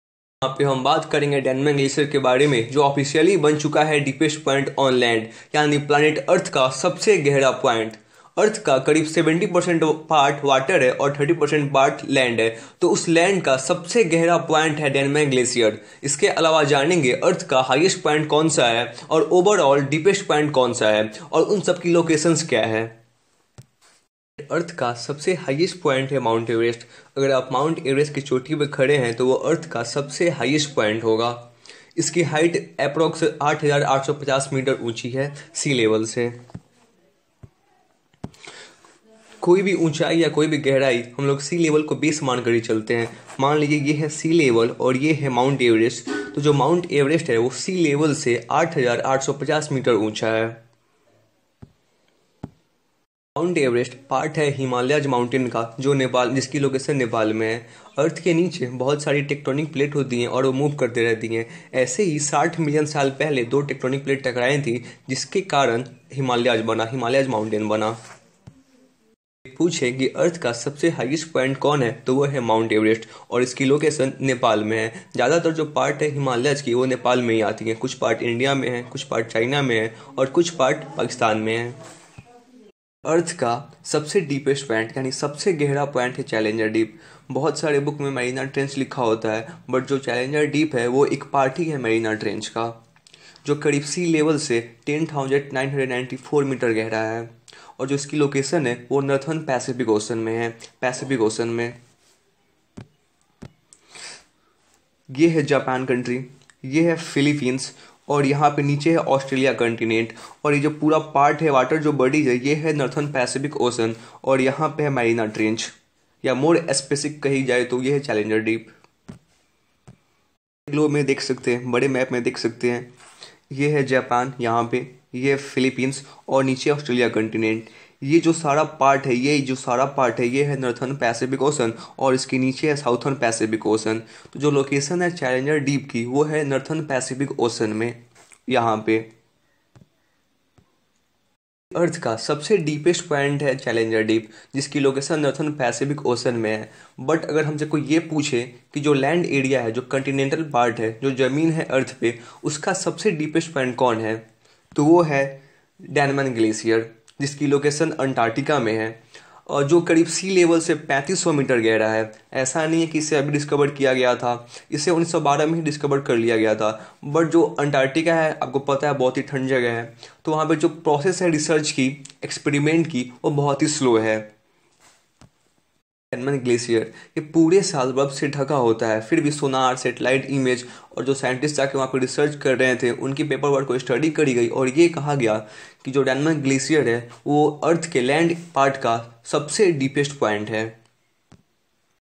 DENMAN Glacier 'deepest point on Earth' पृथ्वी की सबसे गहरी जगह।
Denman Glacier is the deepest point on Land.
It's depth is approx 3.5km